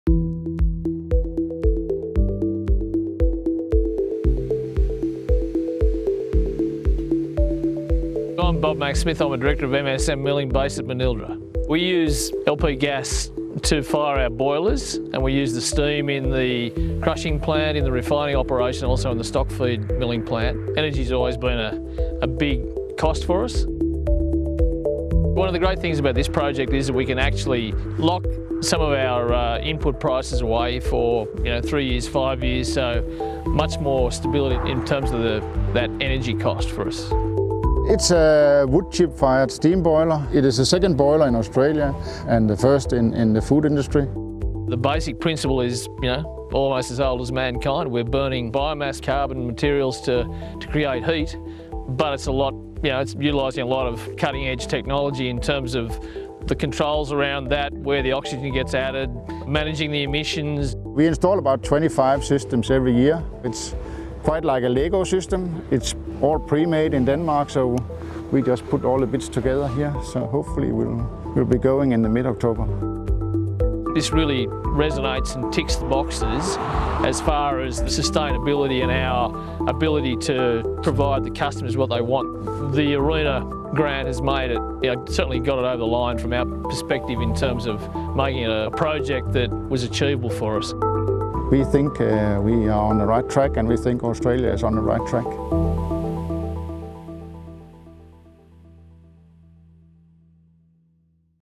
MSM Milling Biomass Fuel Switch Project
The MSM Milling Biomass Fuel Switch Project will replace LPG boilers with a 5MW biomass fuelled boiler using locally sourced timber residue for fuel.